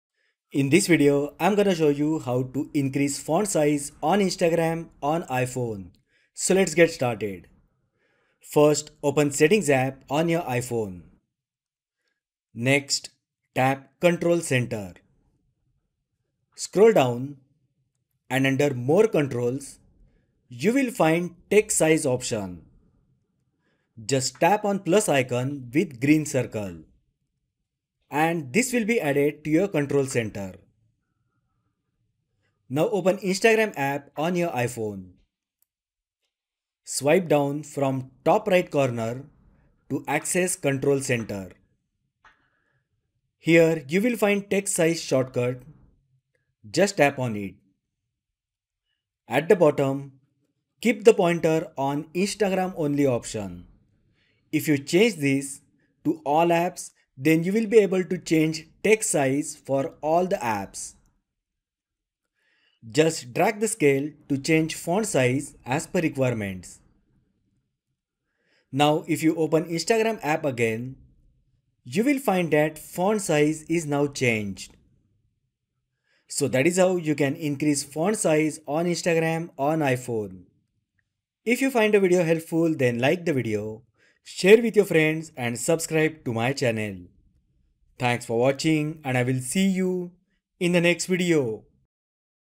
How to Increase Font Size on Instagram on iPhone
How to Increase Font Size on Instagram on iPhone. Are you struggling to read small text on Instagram using your iPhone? In this step-by-step tutorial, we'll show you exactly how to increase the font size on Instagram, making your browsing experience more comfortable and enjoyable.

Whether you're wondering, "How do you make your Instagram text bigger on iPhone?" or "How do you enlarge the font on Instagram?" – we've got you covered. We'll guide you through the process of adjusting the font size to suit your preferences.

Learn how to increase or decrease font size on Instagram on iPhone with ease. Plus, discover a handy trick to change your iPhone's text size for a specific app like Instagram.

Don't strain your eyes any longer – watch this video and start enjoying a clearer, more readable Instagram experience on your iPhone today!